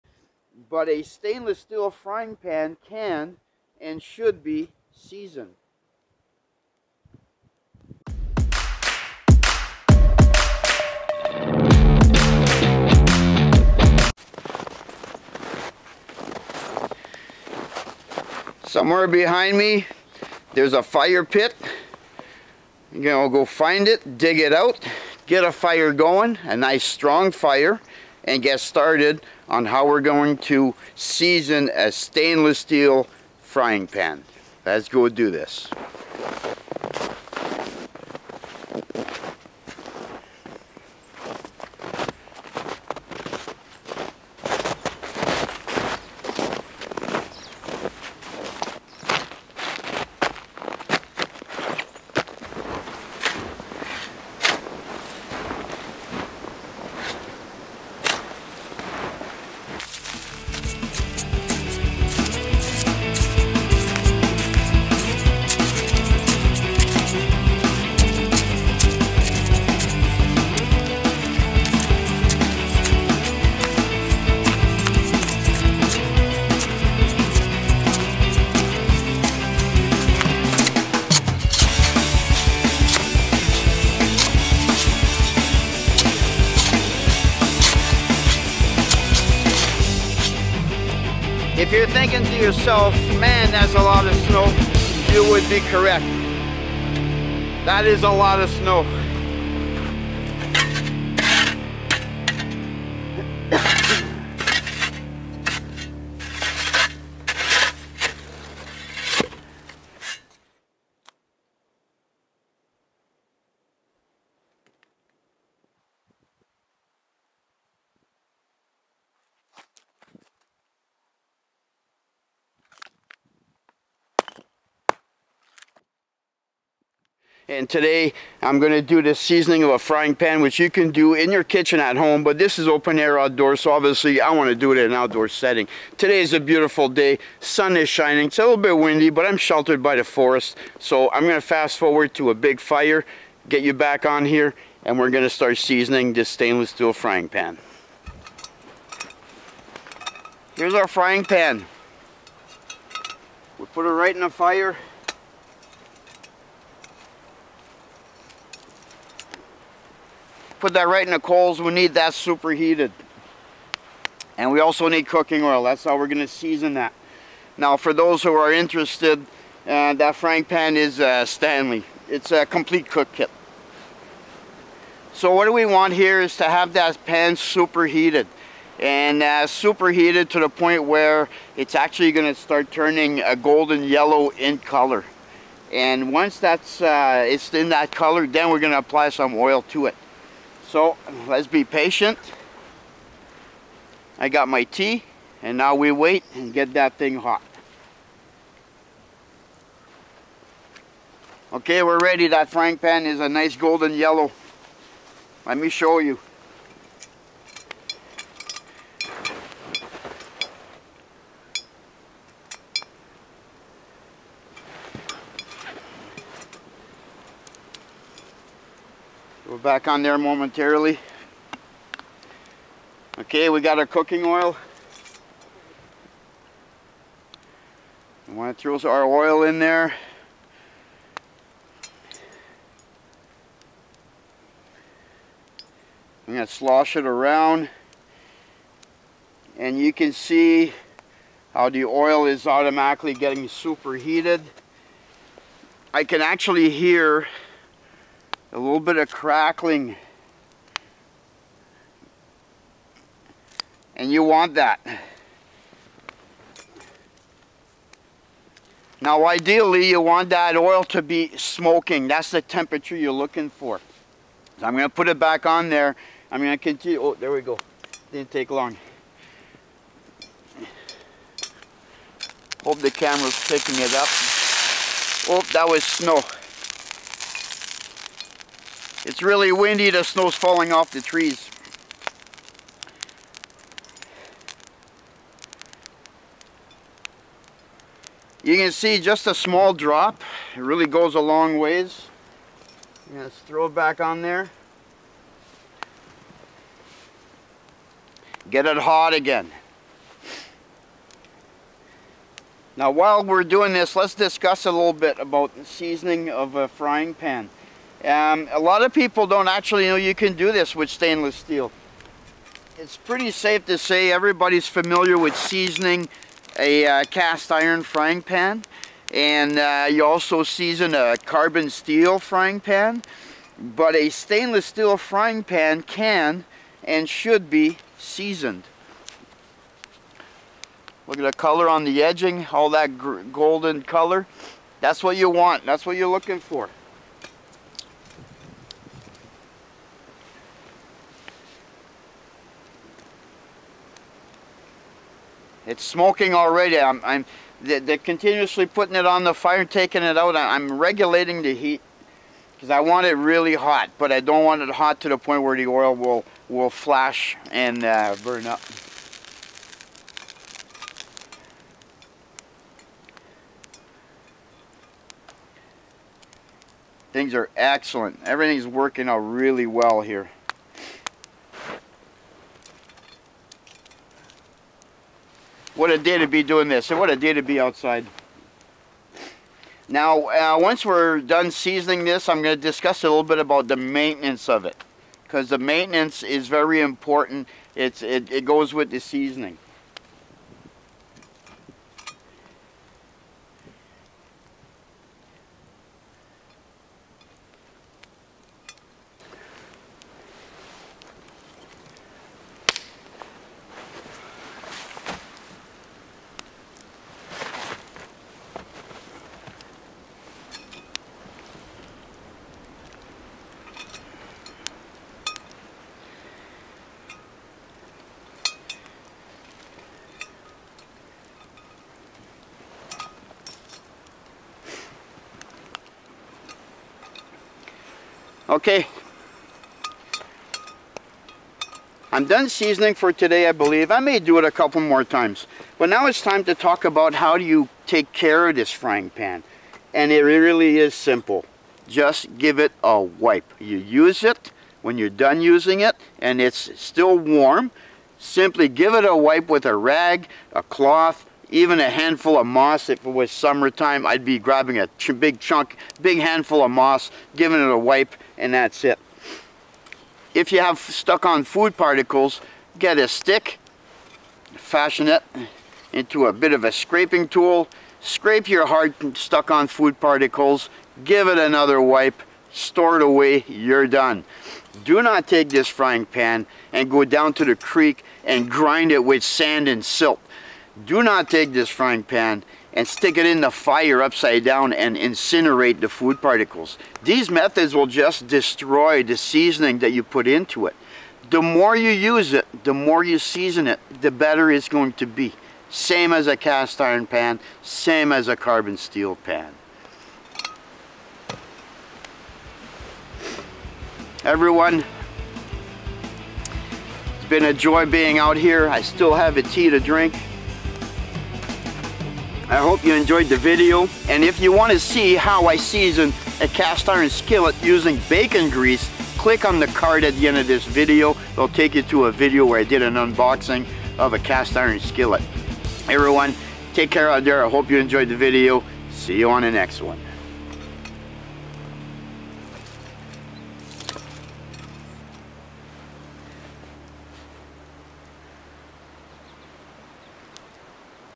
SEASONING stainless steel frying pan, how easy, it is done!
Did you know stainless steel frying pans should be seasoned? They can and should be seasoned, to make they perform better cooking your next meal!
See how it is done in an outdoor setting.